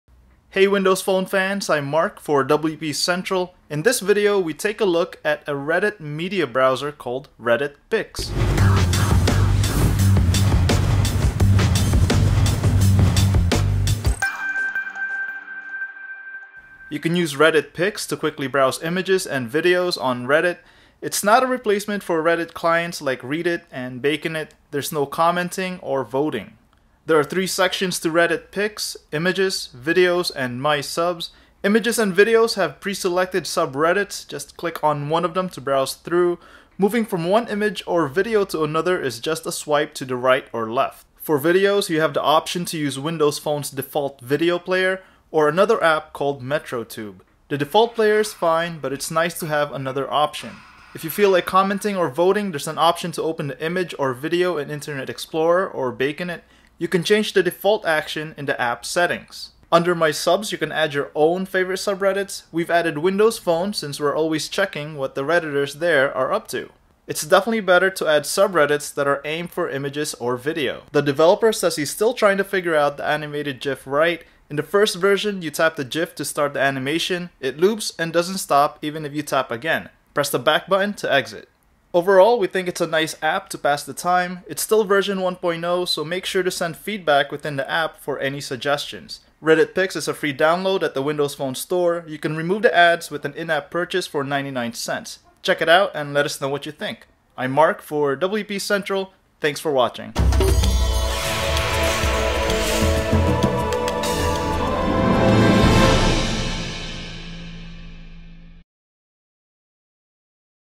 Reddit Pix for Windows Phone 8
With Reddit Pix for Windows Phone 8, you can quickly browse images and videos on Reddit.